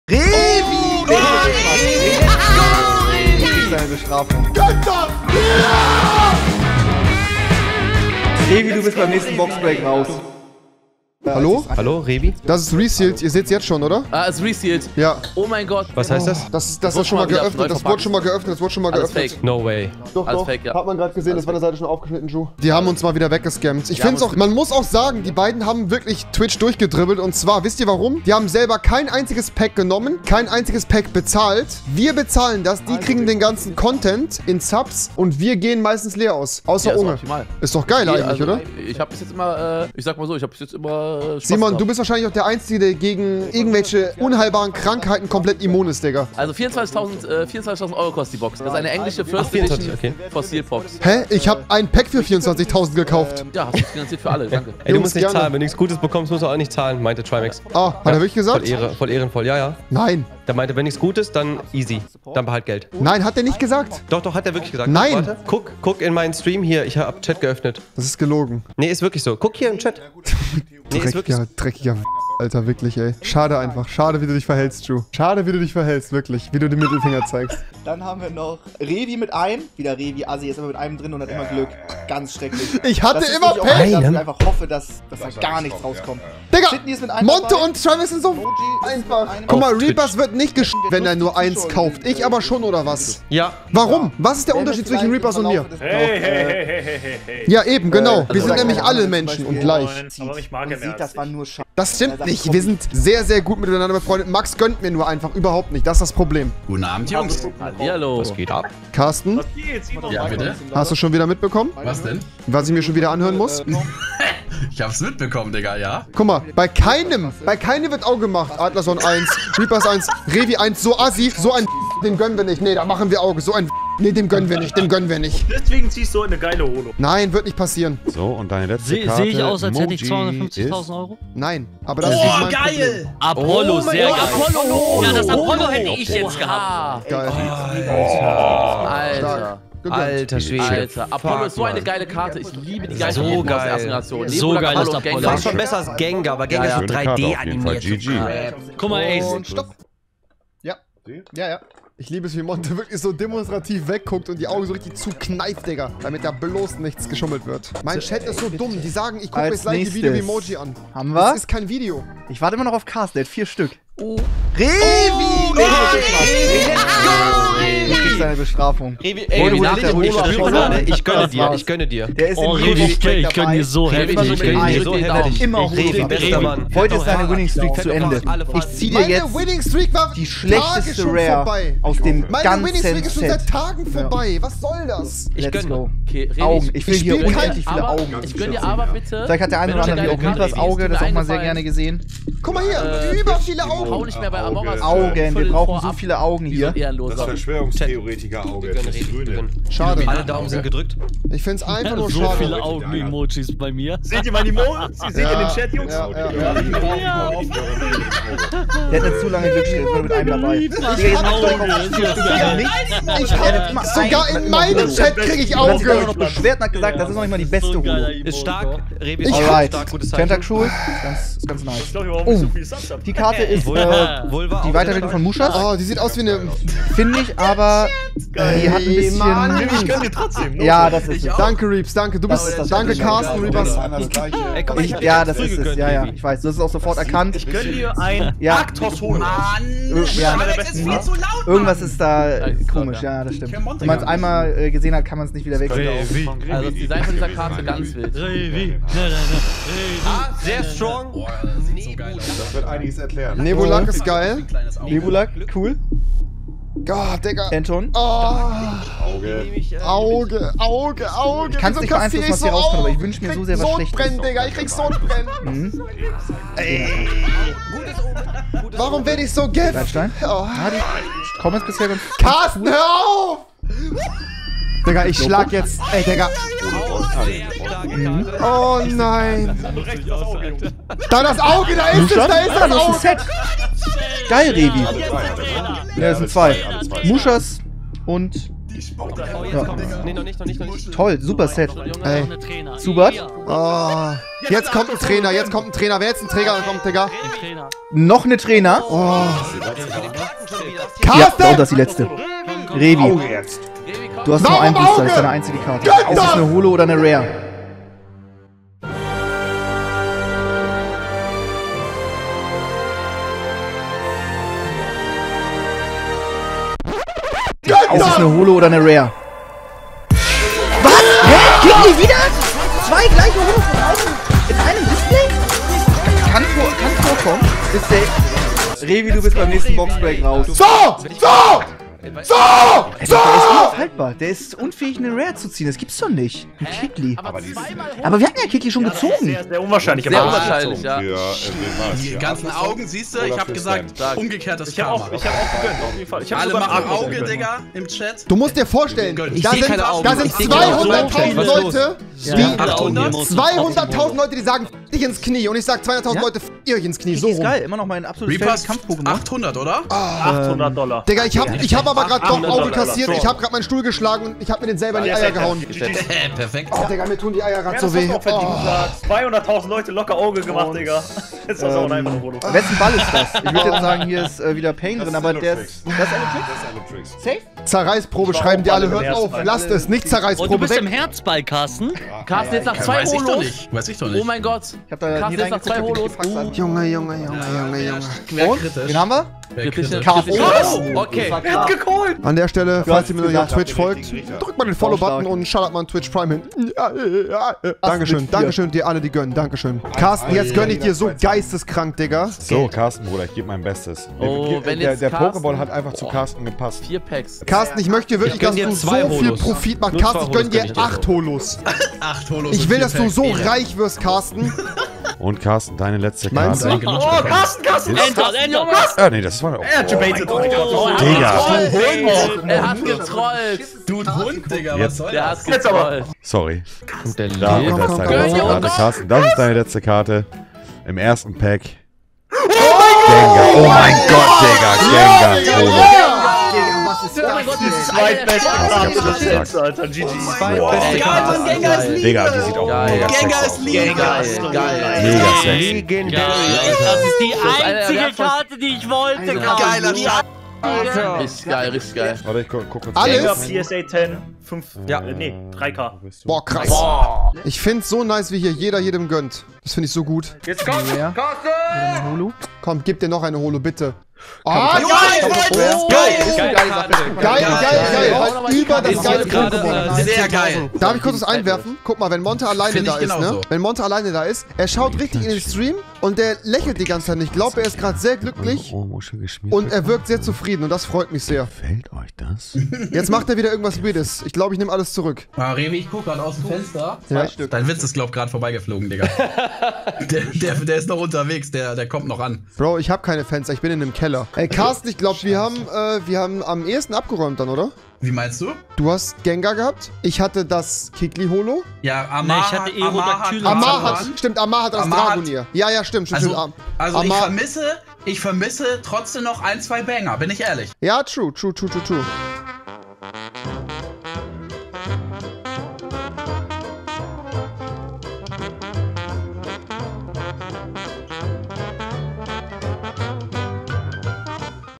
MONTE & Trymacs haben mir NICHT GEGÖNNT & dann ziehe ich diese KARTE! Alle machen 👁!
Ich habe bei Trymacs und Monte im Stream ein Booster gekauft und DAS habe ich bekommen 😍😱
Kuss geht raus an jeden, der Auge gemacht hat 👀